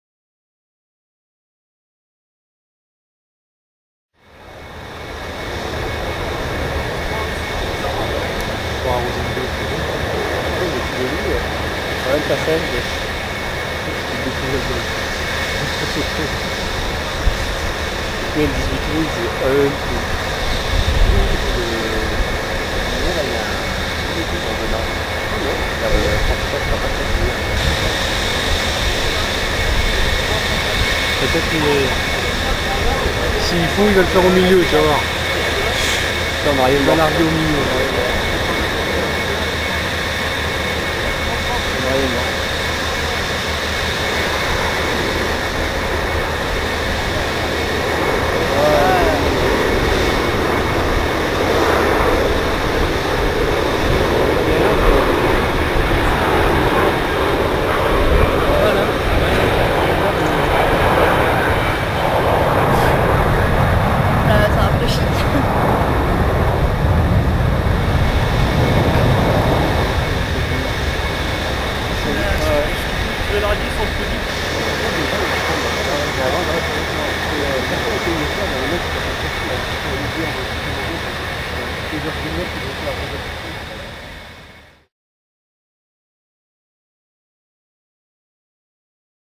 Salon du Bourget 2005 le vol du DC-10 Version tracker.
Salon du Bourget 2005 le vol du DC-10 Version tracker par Harrypilote.